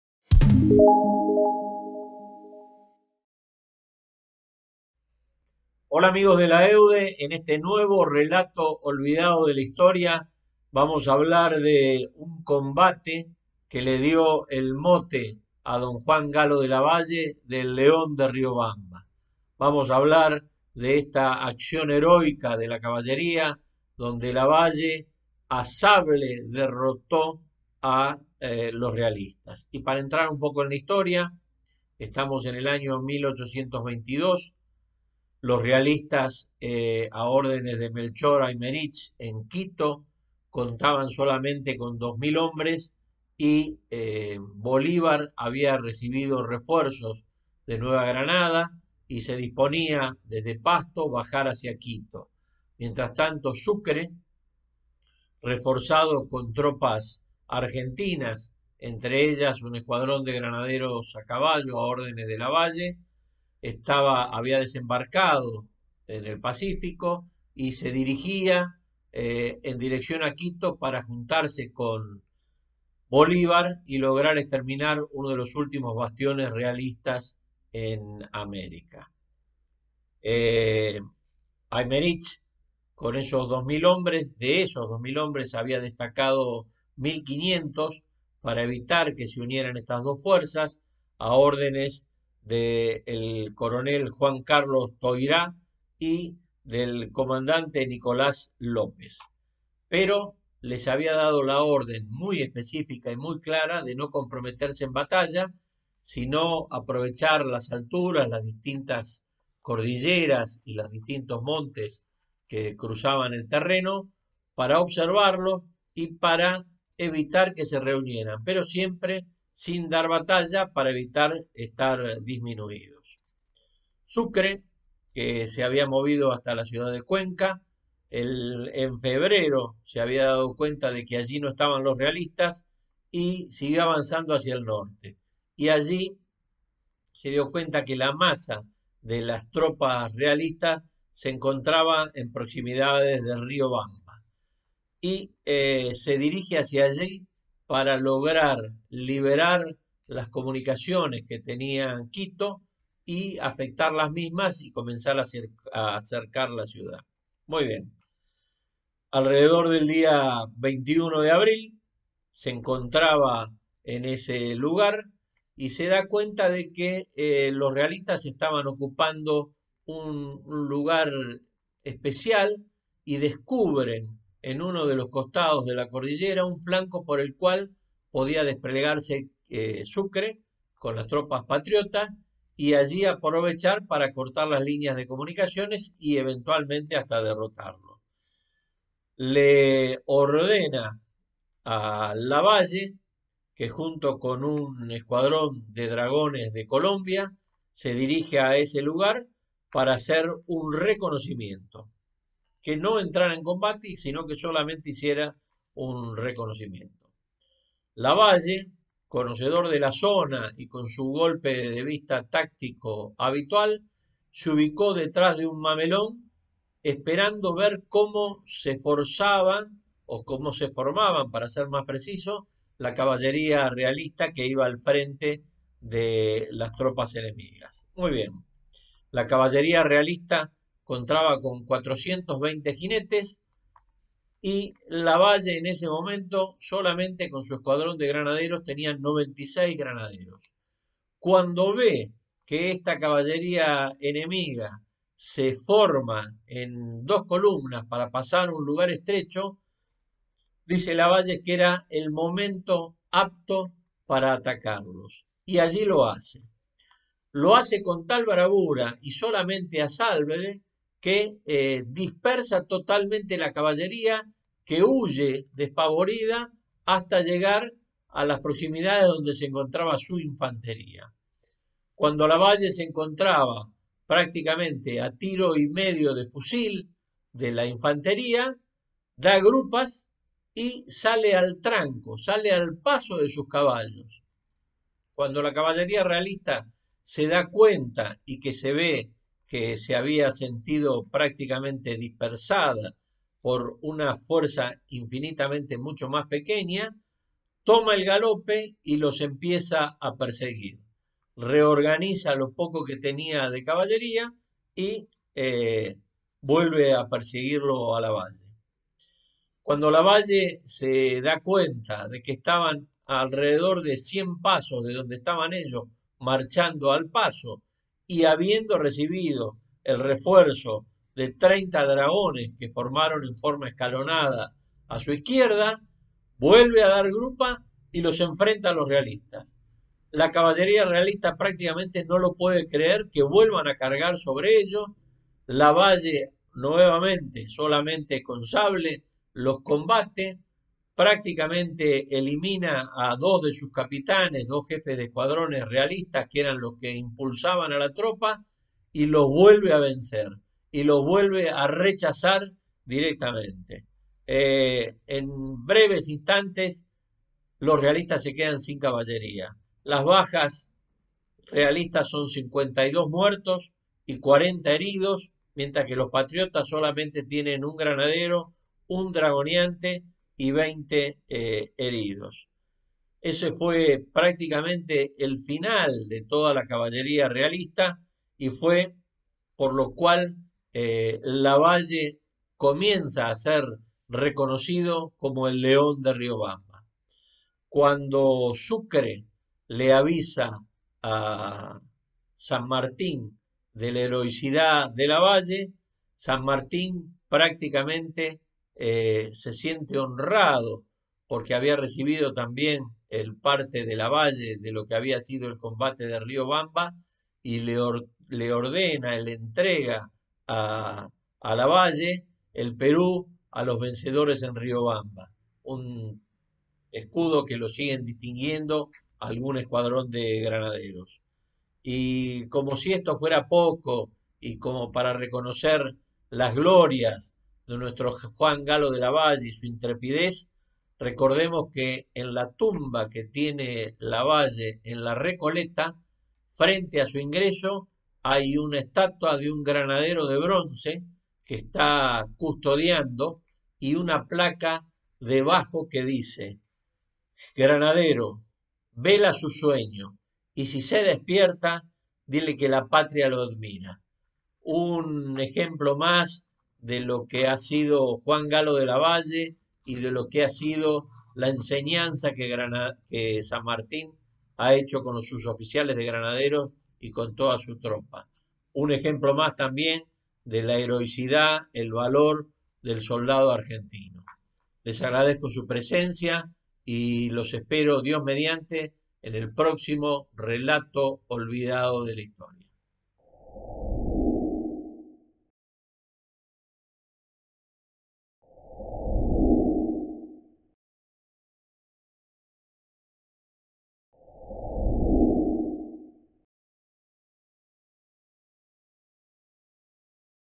Lavalle y el combate de Riobamba
Descubra al genio táctico de Lavalle cuando se retira al paso del combate para separar la caballería realista de la infantería. Cuando lo logra vuelve grupas y carga contra la caballería derrotándola para facilitar la derrota posterior de la infantería. De esta acción recibe el título de "El león de Riobamba".